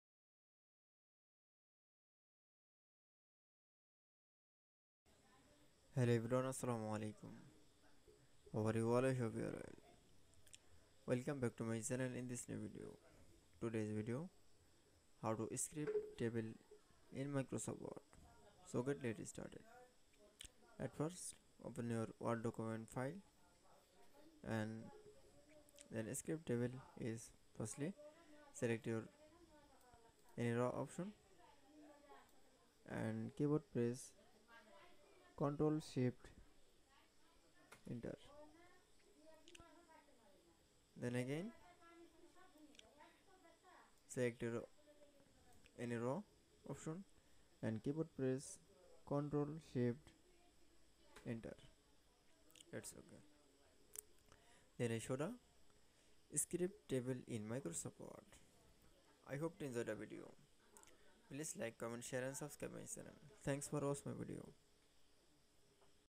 How to Script Tables in Microsoft Word
Assalamu Walaikum,
In this video, I will Present How to Script Tables in Microsoft Word. Let's get Started.